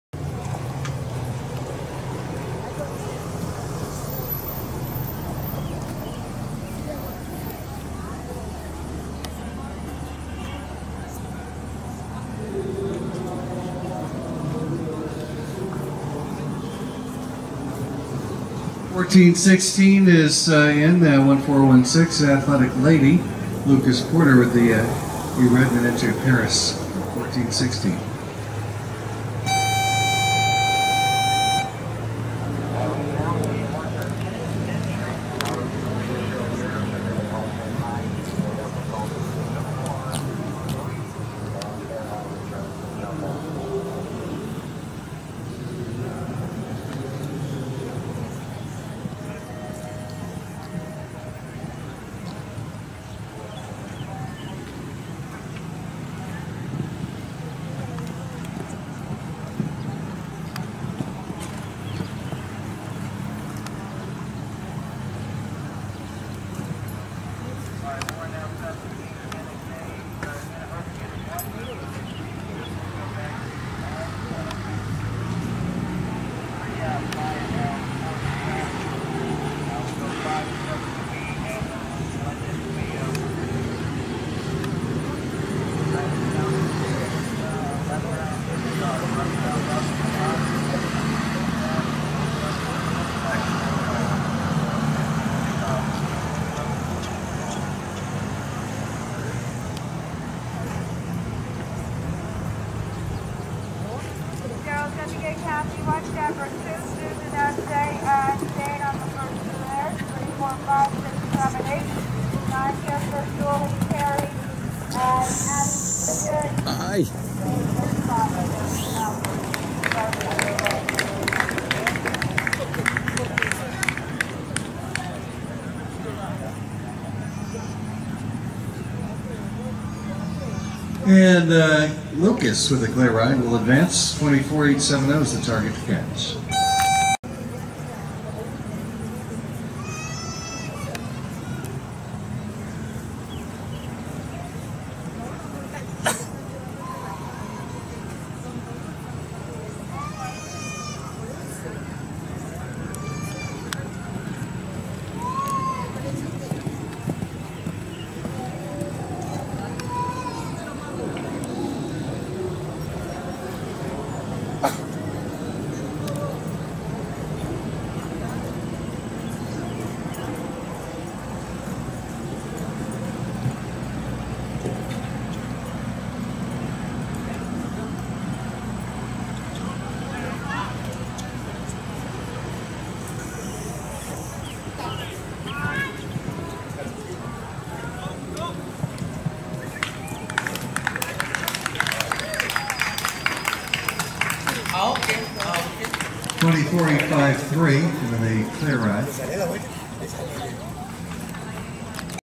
Athletic Lady ridden by Lucas Porter winning Jr. Jumper Classic KY Summer 2
16.2H 9 Yr Old Grey Dutch bred mare